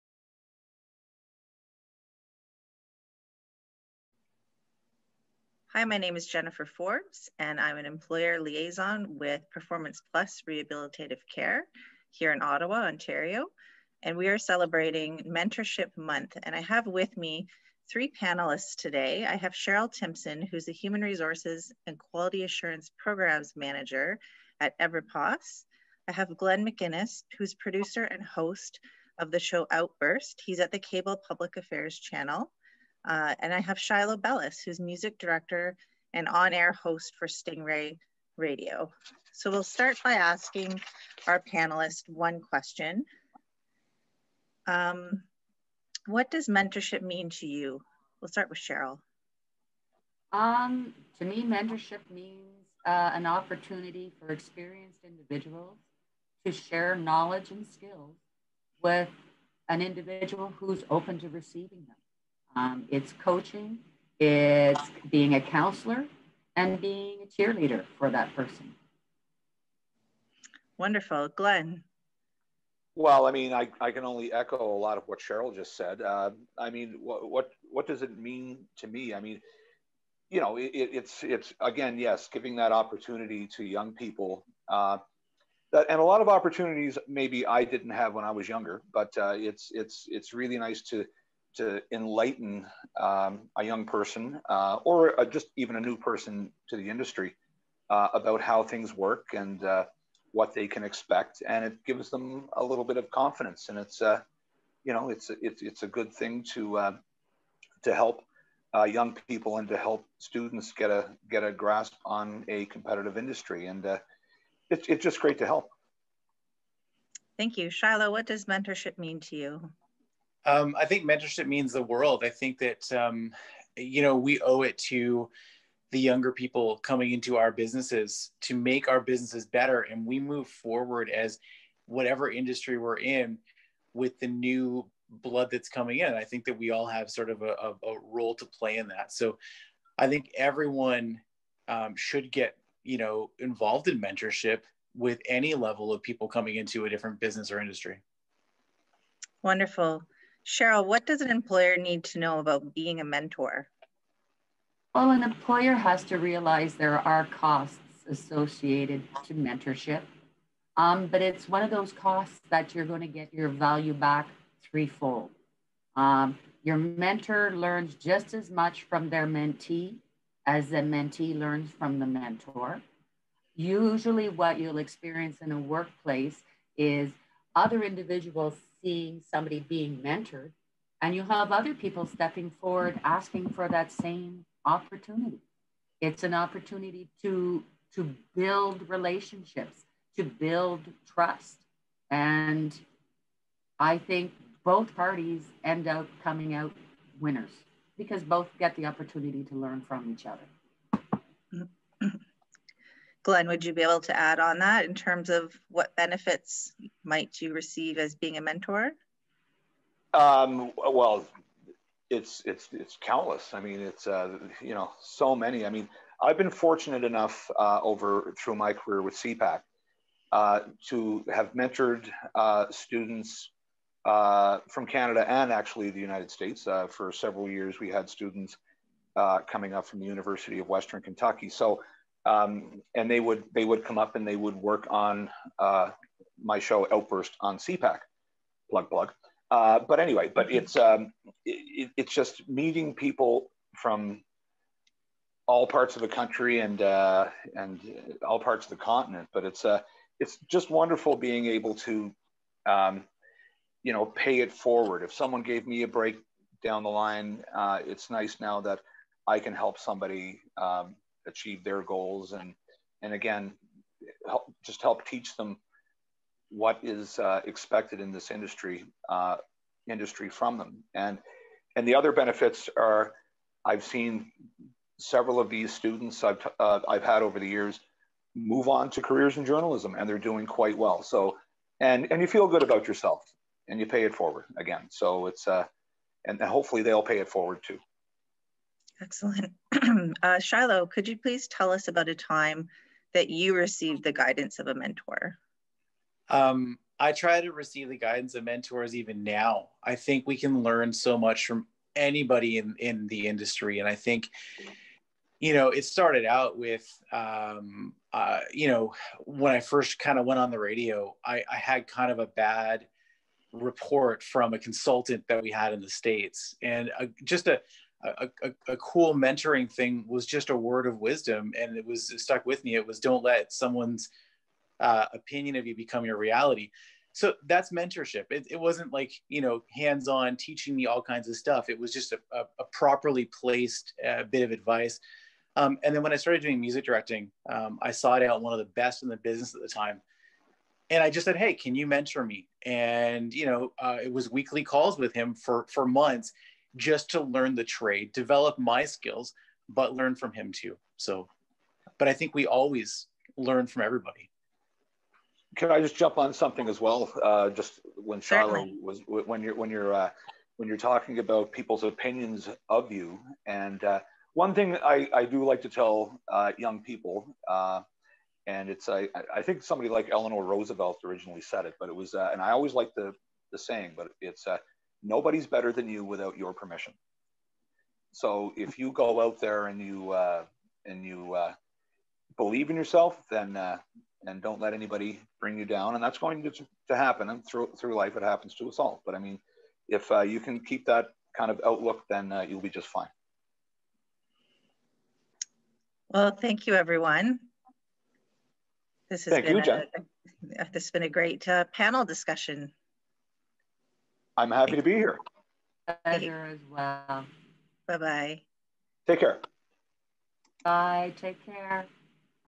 Mentorship Month - PPRC MentorAbility
A panel discussion moderated by Jennifer Forbes of PPRC on the benefits of Mentorship.